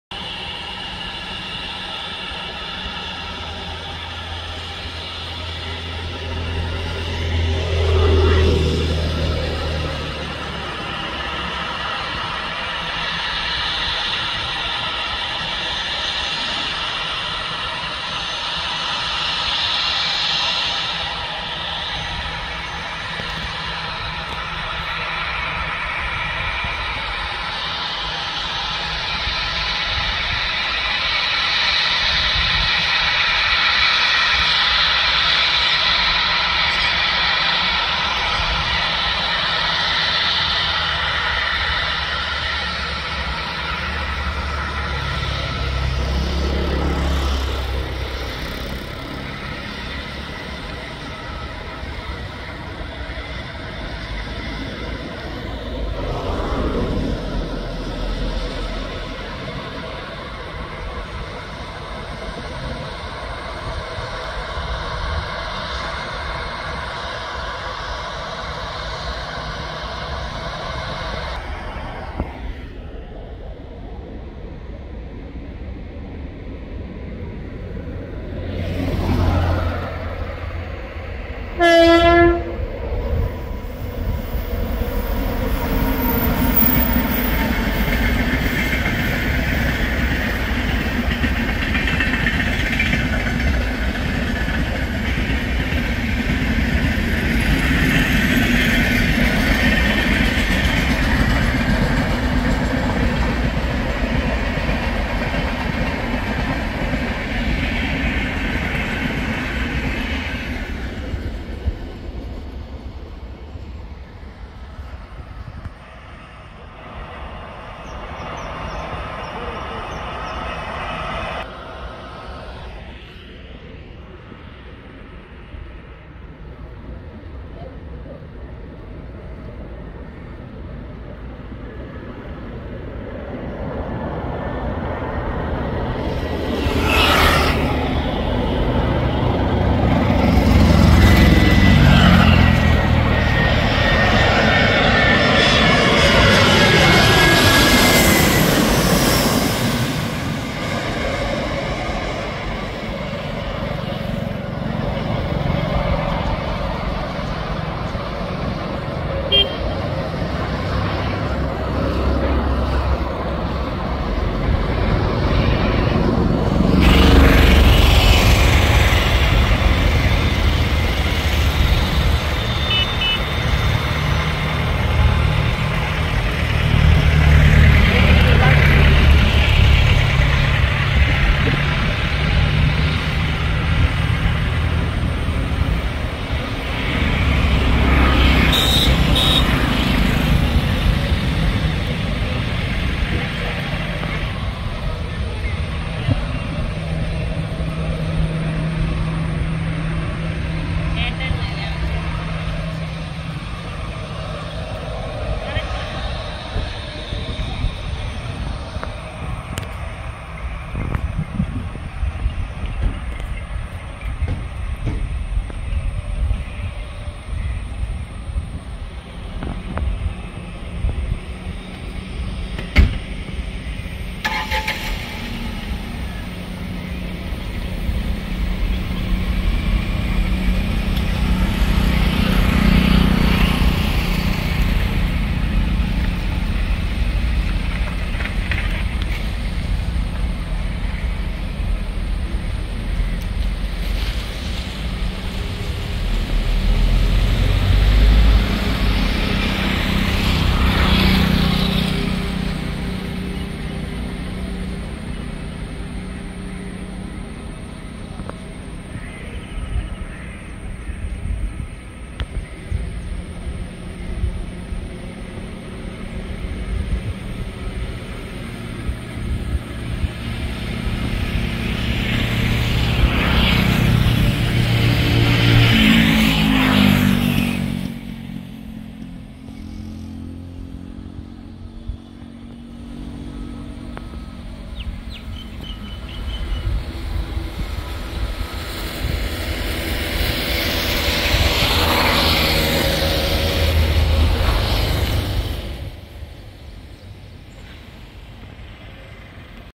Landing Of AIR ASIA | Take Off of AIR INDIA | PASSING OF TRAIN | DHANBAD EXPRESS | COCHIN AIRPORT
നെടുമ്പാശേരിയിൽ അല്പനേരം.. indianrailways airasia airindia plane aeroplane #2023